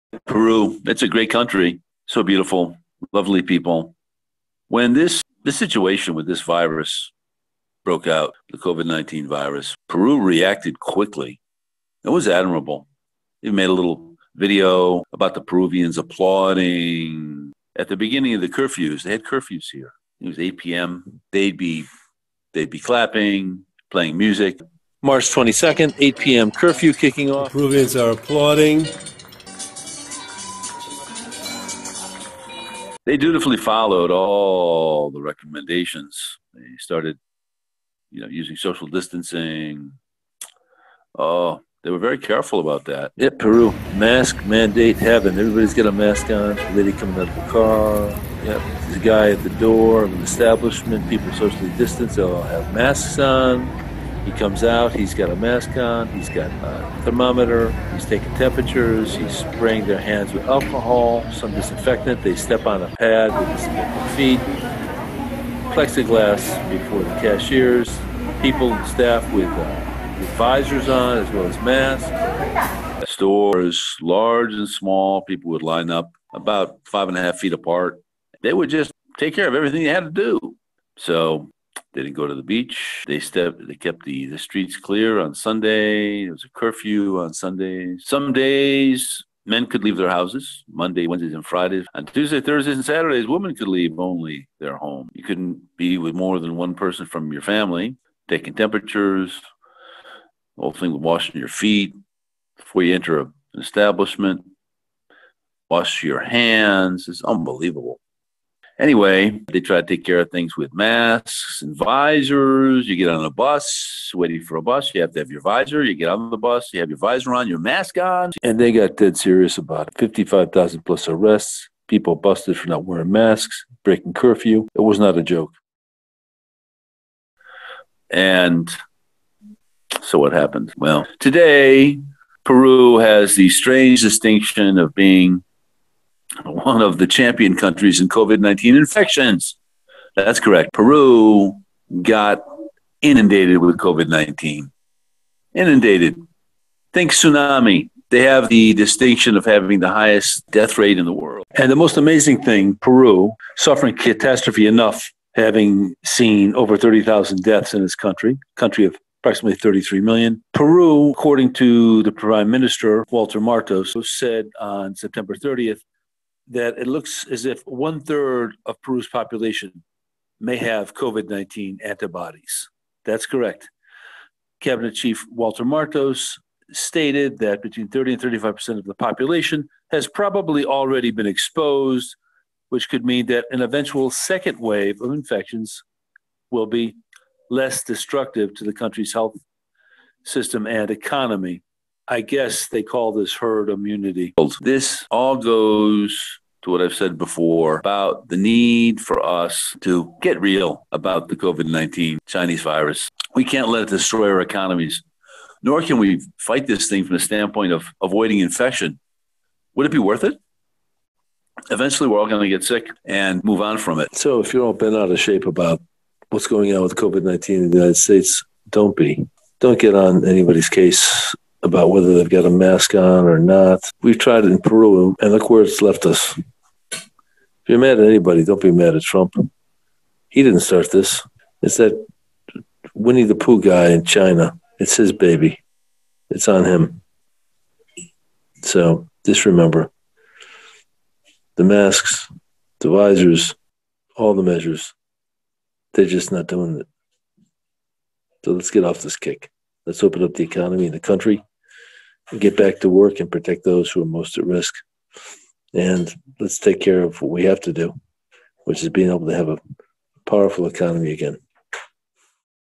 QUORUM RADIO America at a Crossroads-Will we Learn from Peru's Debacle?
Peru Employed Superior Measures to contain the Covid 19 Virus. Lets see how it worked out.